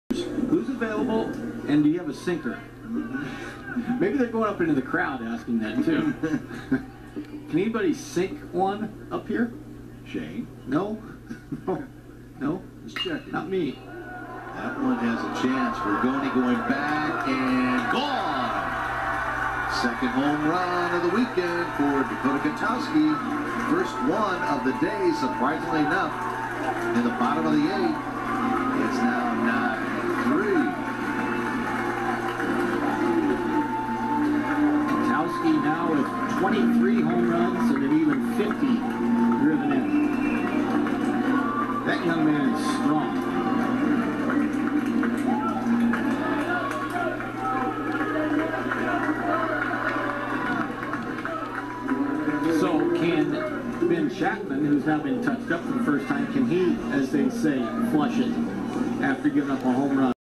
Missouri State baseball's Dakota Kotowski hits 23rd home run of year vs. Southern Illinois
Missouri State baseball's Dakota Kotowski hits a homer, his 23rd of the season, vs. Southern Illinois in the conference tournament, 5/29/22.